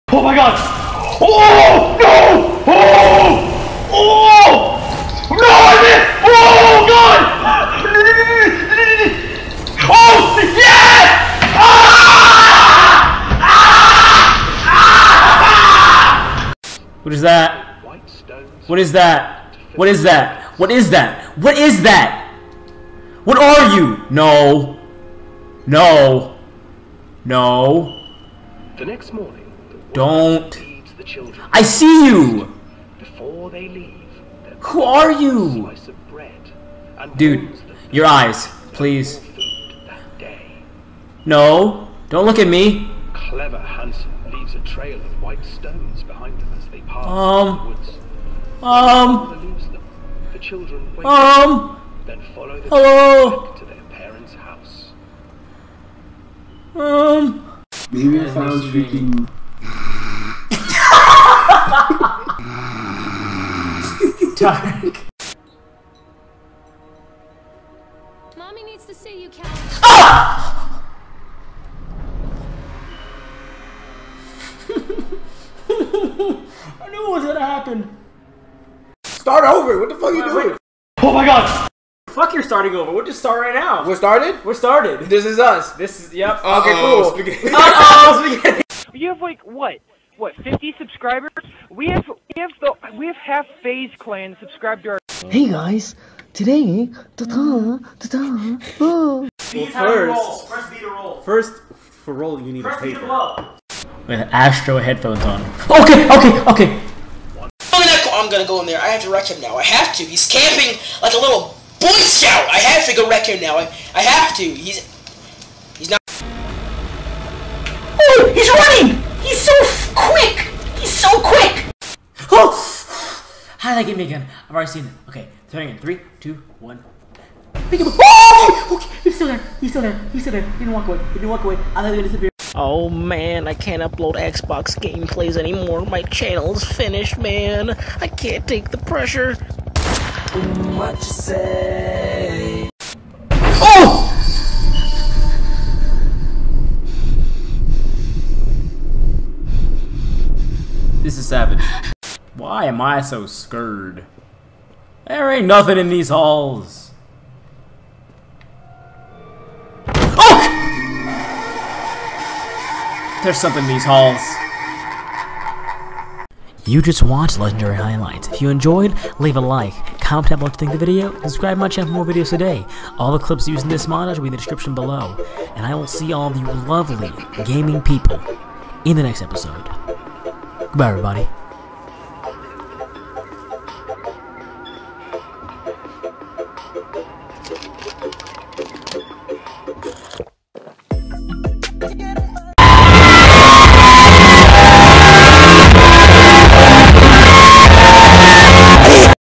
Legendary Highlights #3 | 11,000 View Special
Thank you all so much for 11,000 views...this means the world to me. I've been working on this highlight video for about a week now, and I know it's not much, but I wanted to do a special kind of video, something out of the norm, to kind of say thanks. I appreciate all the support from you guys and other channels that have helped me. I even threw in a few old videos as a sort of look back on where we started and how much we've changed since. I'm still working on finding a new series to start for you guys, if you have any ideas, please comment, but otherwise I hope you guys enjoy this video, thank you all once again, and I'll see you all later

Feeling drained?